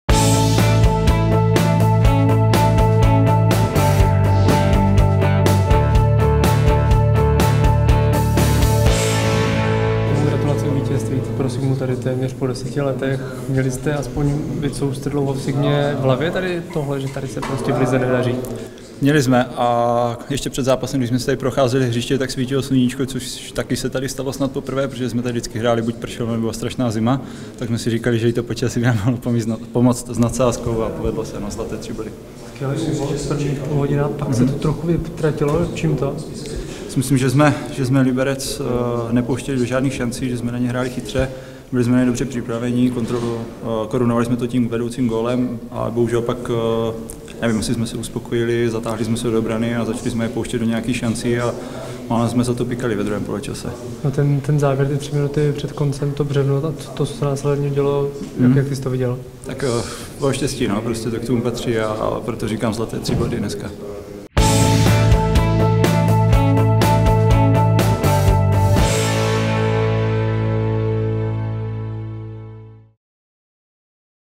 Jakub Plšek po utkání s týmem FC Slovan Liberec
Záložník Jakub Plšek hodnotí vítězné utkání 4. kola FORTUNA:LIGY s týmem FC Slovan Liberec